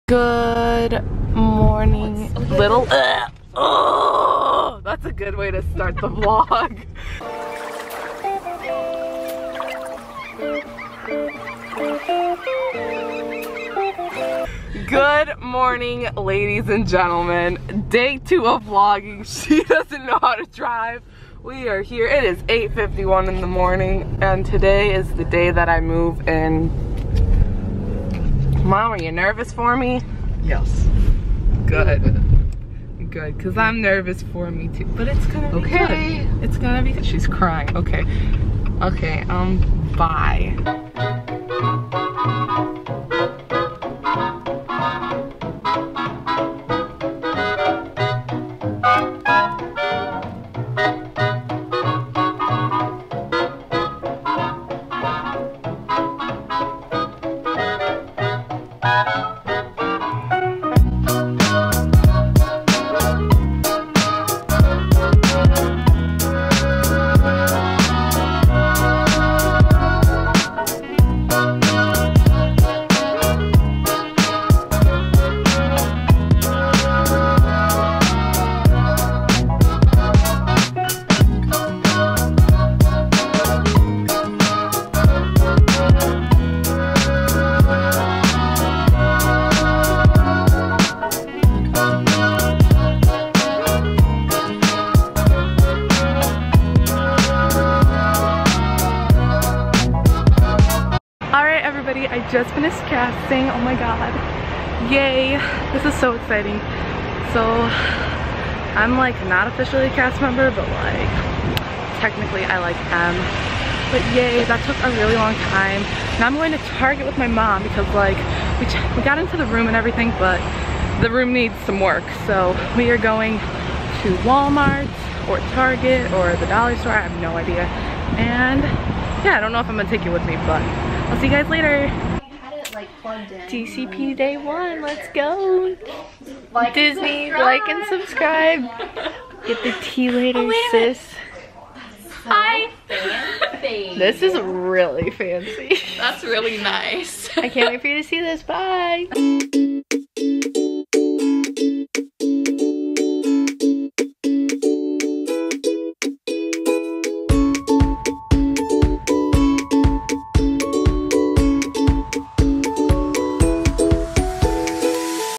CHECK IN AND CASTING | dcp spring 2019: vlog 2
Wow... I'm so cool... just kidding. But yay I finally moved into the DCP!